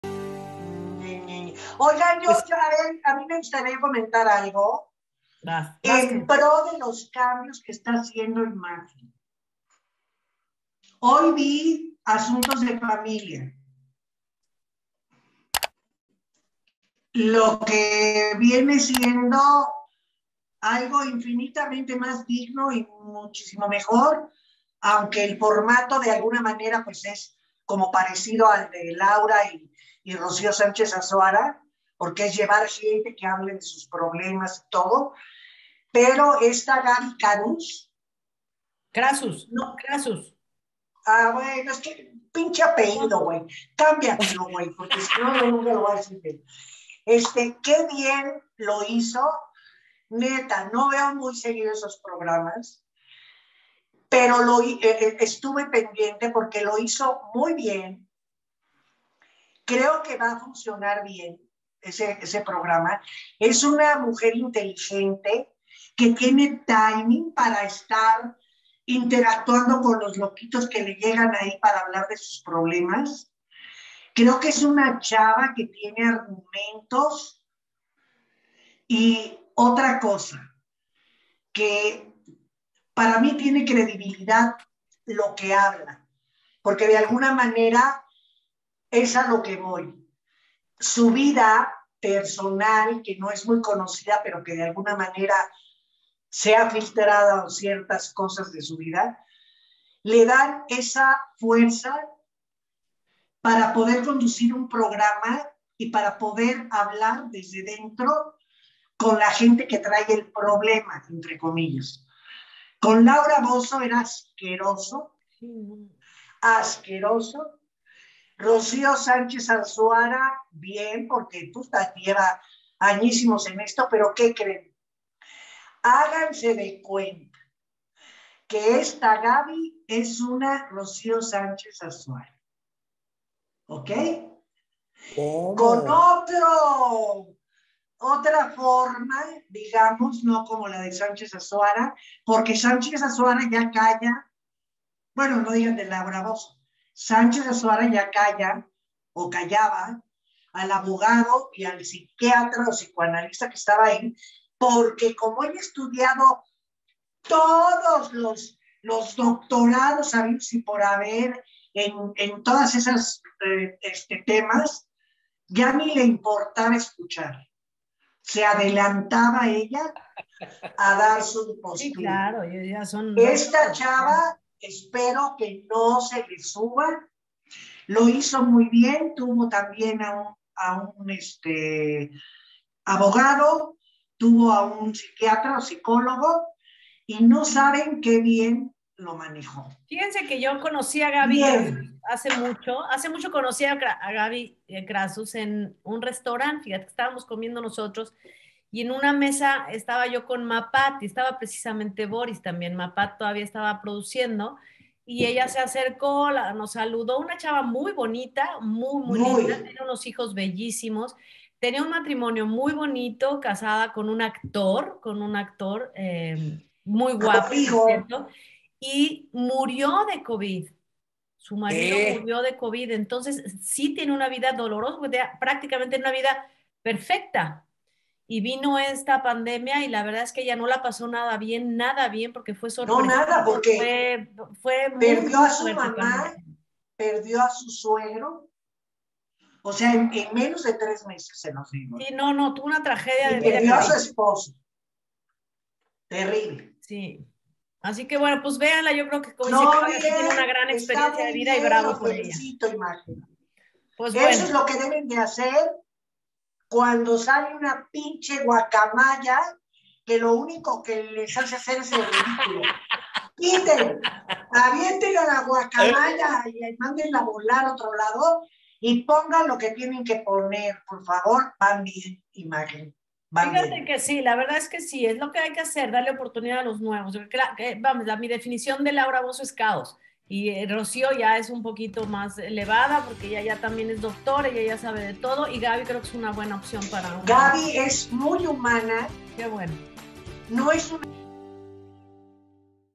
Gaby Crassus en lugar de Laura Bozzo.
Programa de opinión sobre las notas más destacadas del entretenimiento y la vida en general.  con Lupita Martínez, Claudia de Icaza, Poncho Martínez, Aurora Valle y Luis Magaña